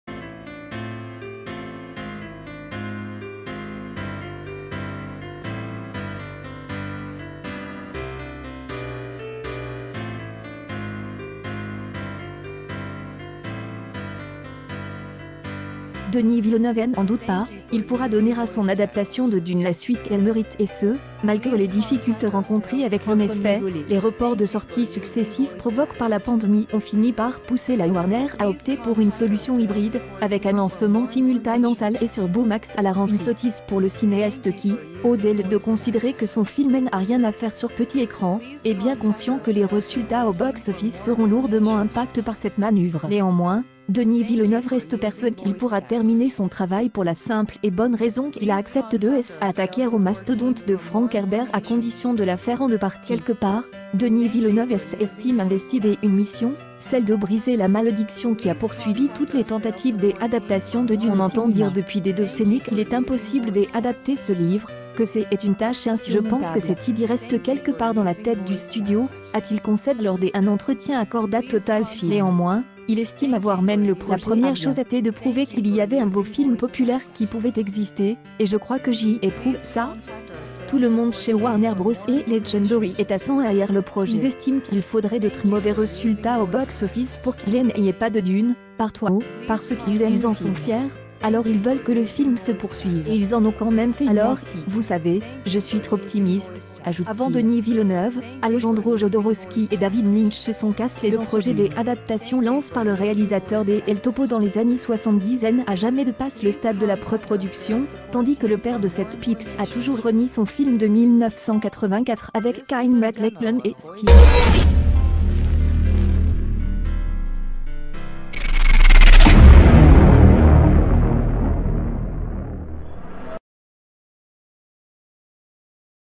Denis Villeneuve pense que « Dune part 2 » est en bonne voie même s’il a des doutes
Denis Villeneuve pense que « Dune part 2 » est en bonne voie même s’il a des doutes Denis Villeneuve n’en doute pas, il pourra donner à son adaptation de Dune la suite qu’elle mérite et ce, malgré